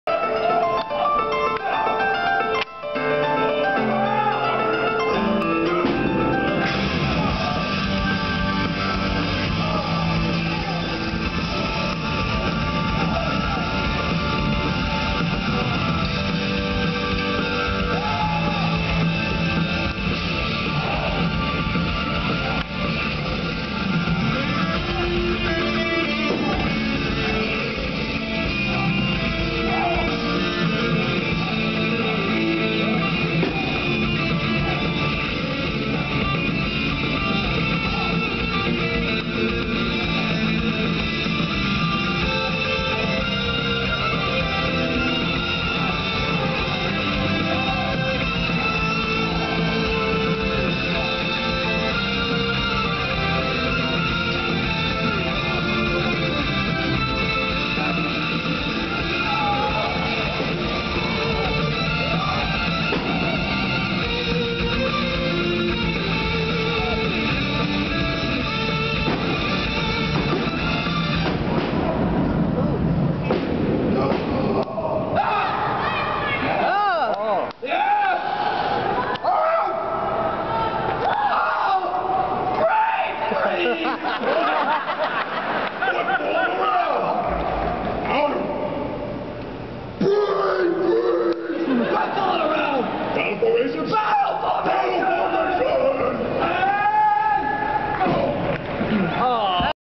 METRO10 Warriors vs SubZero
Red and Yellow Warriors from Gauntlet
VS
SubZero from Mortal Kombat

Anime Human Chess Match, METROcon 2010